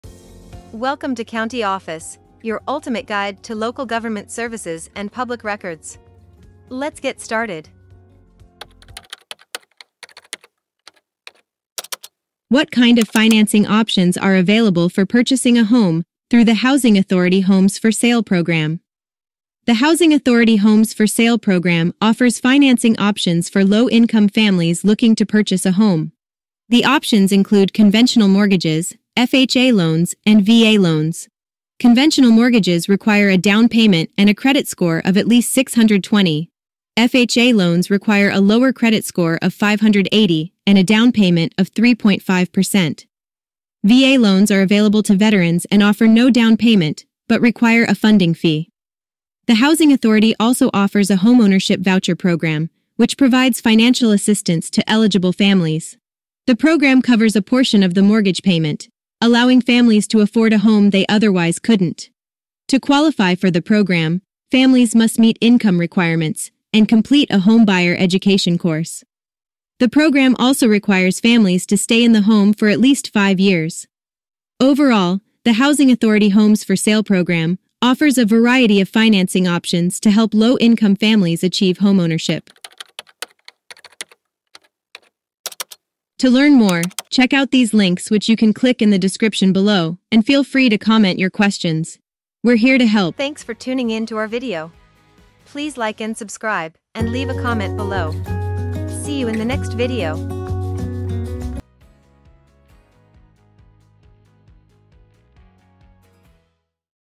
What Financing Options for Housing Authority Homes for Sale? - CountyOffice.org
What Kind Of Financing Options Are Available For Purchasing A Home Through The Housing Authority Homes For Sale Program? Are you a low-income family looking to purchase a home? The Housing Authority Homes for Sale Program offers a variety of financing options to help you achieve homeownership. From conventional mortgages to FHA and VA loans, there are options available to fit your needs. The Homeownership Voucher Program also provides financial assistance to eligible families, covering a portion of the mortgage payment to make homeownership more affordable. To qualify, families must meet income requirements and complete a homebuyer education course.

In this video, we'll explain everything you need to know about financing options available through the Housing Authority Homes for Sale Program. We'll discuss the requirements for conventional mortgages, FHA loans, and VA loans, as well as the Homeownership Voucher Program. We'll also provide helpful resources for those looking to learn more about the program and how to apply.

for more information on the Housing Authority Homes for Sale Program and other housing resources. You can also visit HUD.gov and FHA.com for additional resources on buying a home.